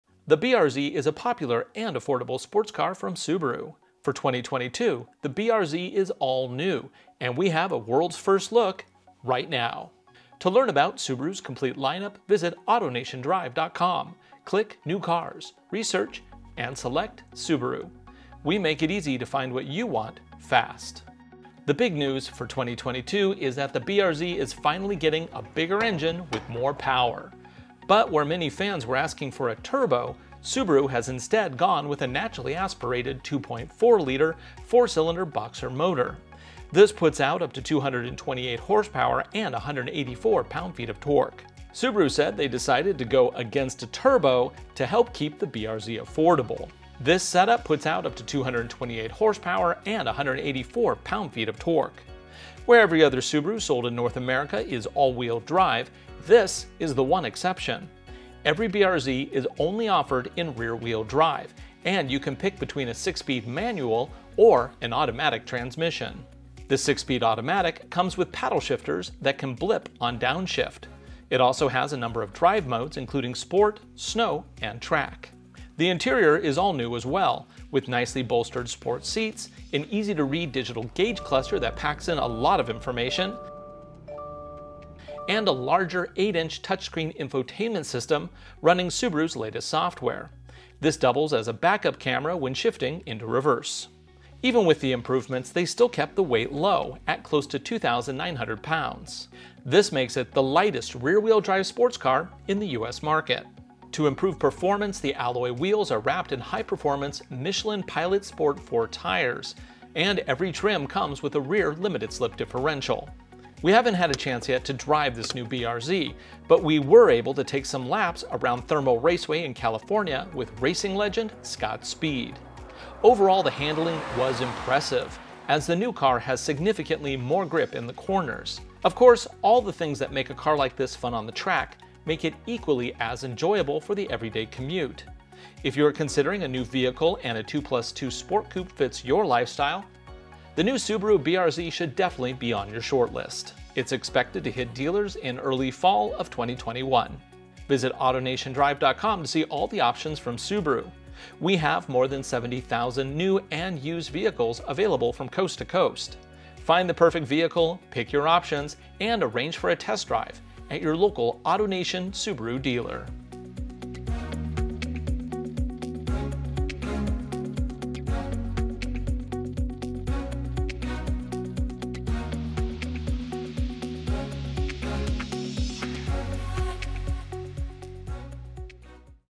AutoNation presents the First Look at the 2022 Subaru BRZ
The BRZ is a popular and affordable sports car from Subaru. For 2022, the BRZ is all-new, and we have a world’s first look -- right now.

Dealers, Service and Repairs, coast to coast!